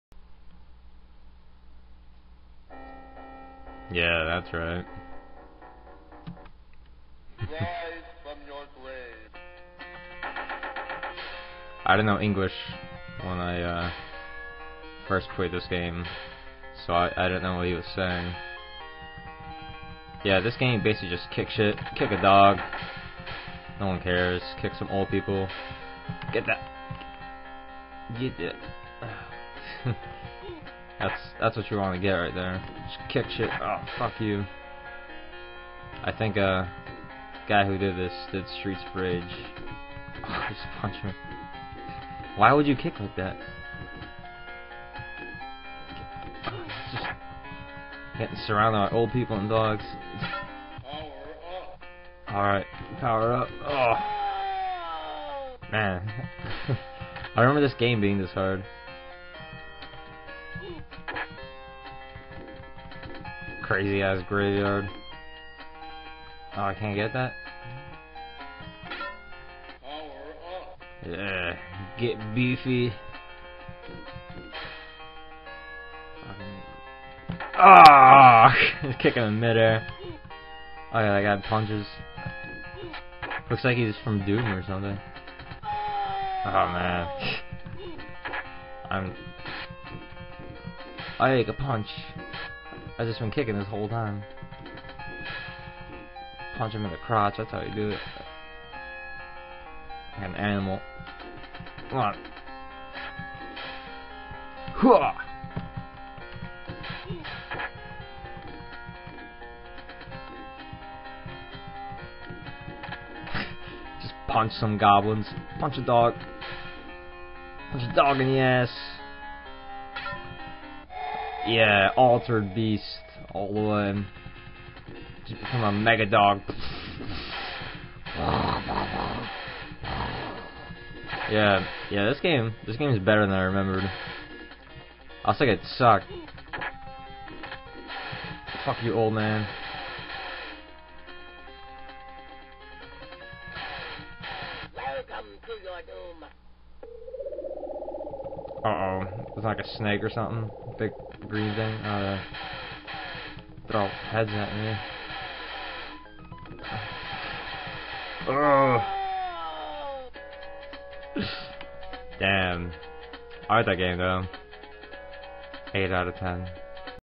Altered Beast: Stoned Review
Classic blaze-em up 3rd person sidescrolling punching and kicking action/adventure arcade classic!